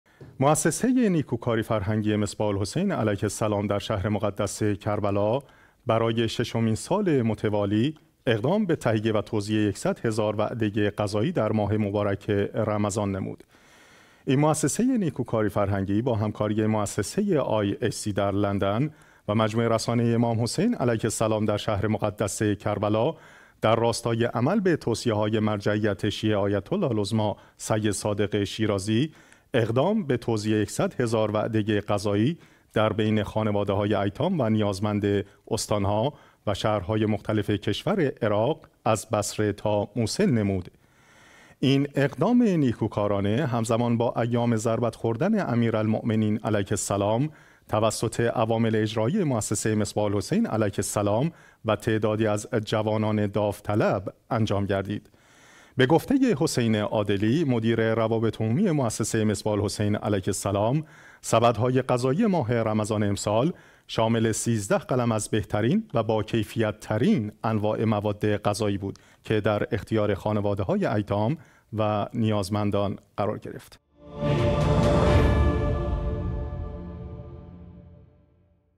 توزیع ۱۰۰ هزار وعده غذایی در کشور عراق توسط موسسه مصباح الحسین
مؤسسه نیکوکاری-فرهنگی مصباح الحسین علیه السلام در شهر مقدس کربلا، برای ششمین سال متوالی، اقدام به تهیه‌ و توزیع ۱۰۰ هزار وعده غذایی در ماه مبارک رمضان نمود.
این موسسه ی نیکوکاری- فرهنگی، با همکاری موسسه  ihc در لندن و مجموعه رسانه ای امام حسین علیه السلام در شهر مقدس کربلا، در راستای عمل به توصیه های مرجعیت شیعه«آیت الله العظمی سید صادق شیرازی»، اقدام به توزیع  ۱۰۰ هزار وعده غذایی در بین خانواده‌های ایتام و نیازمند استان‌ها و شهرهای مختلف کشور عراق، از بصره تا موصل، نمود.
این اقدام نیکوکارانه، همزمان با ایام ضربت خوردن امیرالمومنین علیه السلام، توسط عوامل اجرایی موسسه مصباح الحسین علیه السلام و تعدادی از جوانان داوطلب انجام گردید.
به گفته ی حسین عادلی «مدیر روابط عمومی موسسه مصباح الحسین علیه السلام»، سبدهای غذایی ماه رمضان امسال، شامل ۱۳ قلم از بهترین و با کیفیت‌ترین انواع مواد غذایی بود که در اختیار خانواده های ایتام و نیازمندان قرار گرفت.